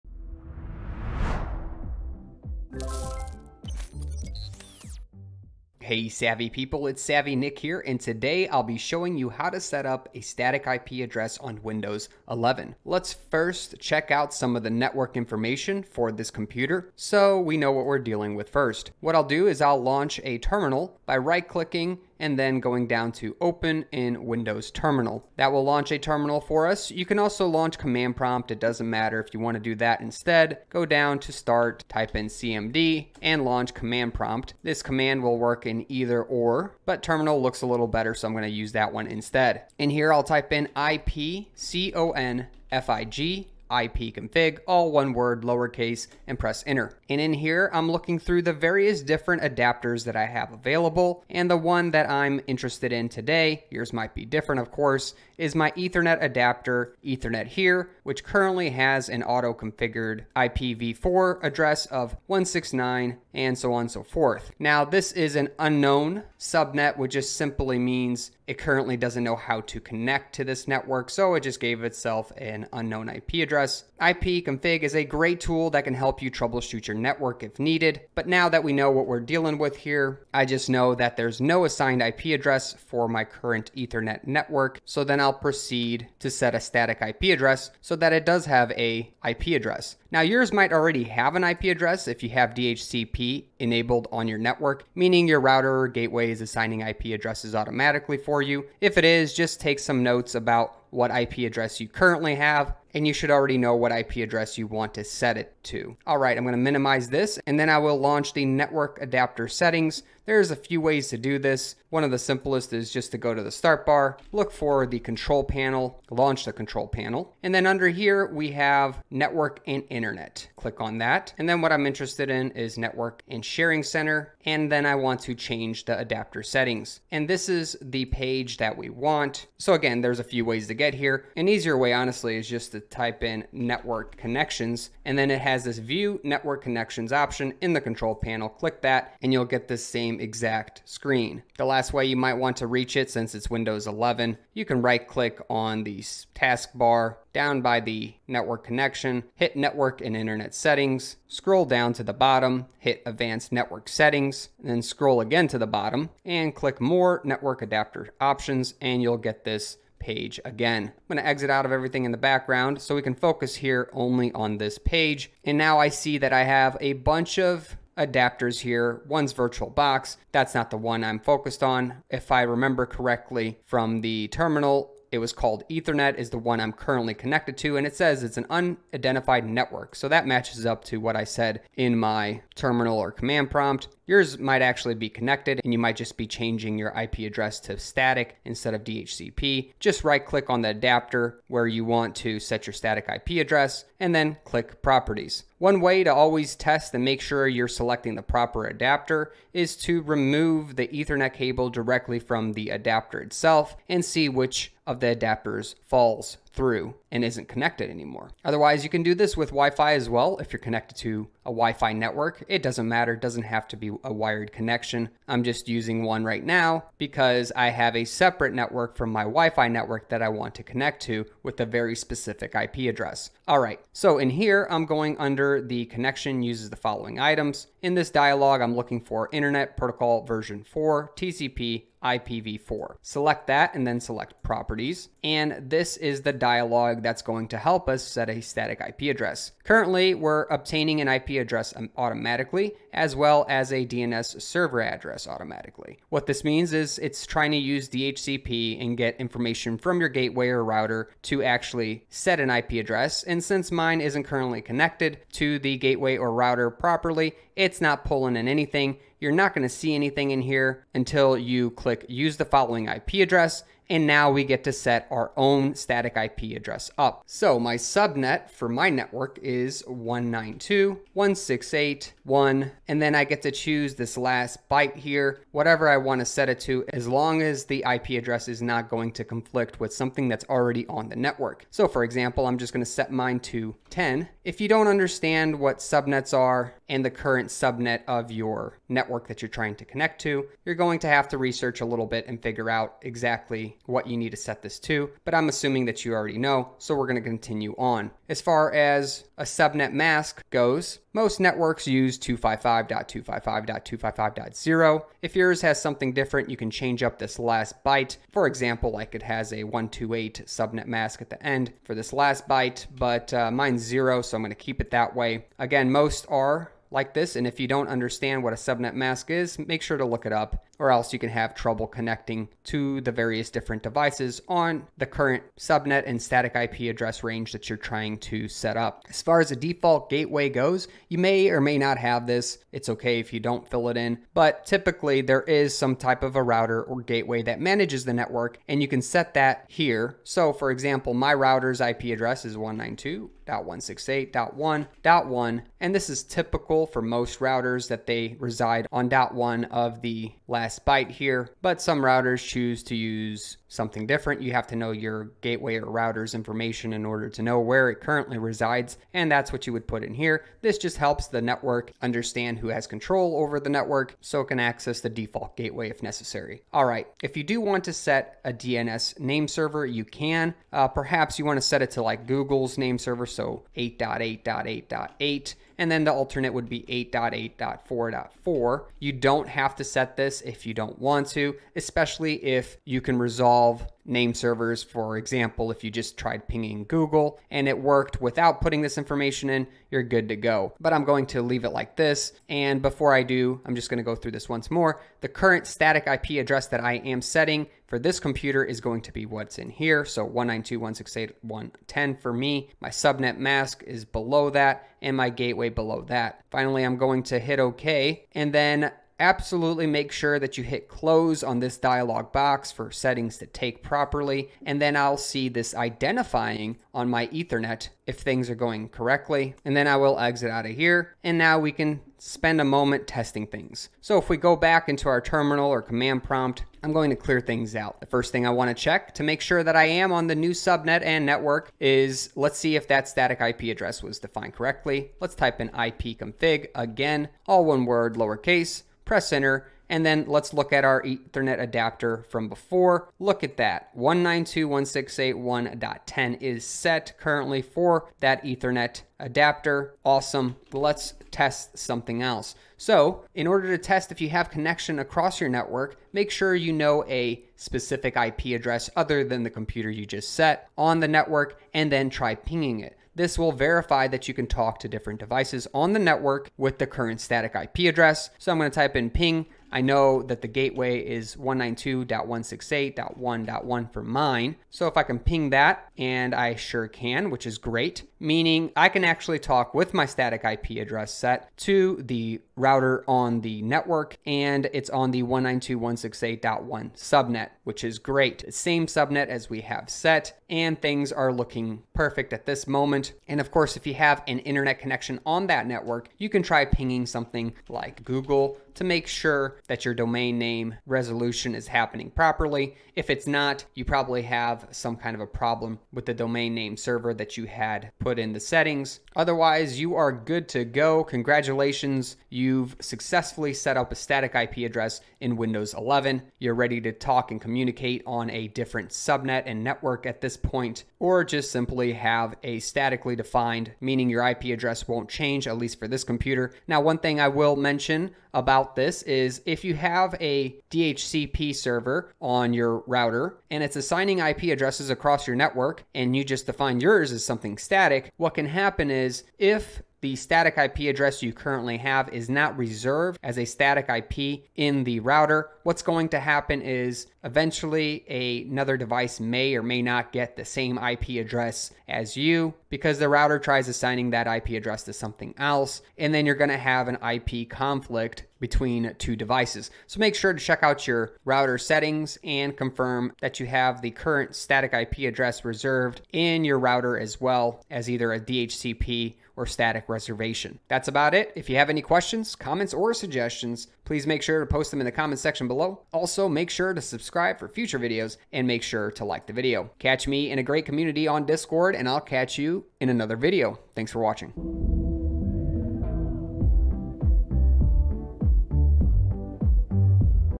How to Setup a Static IP Address on Windows 11 using Network and Adapter Settings.
This tutorial will show you how to set a Static IP Address on Windows 11 using the Network and Adapter settings. Also figure out how to confirm that your Static IP Address has been set in Windows 11 and then figure out how to talk to other devices on the network.

00:00 Finding Network Info
01:46 Network Adapter Settings
03:10 Accessing Network Config
03:58 Setting Static IP Address
06:32 Confirm Settings
07:00 Testing New Static IP
08:47 Router/Network Mentions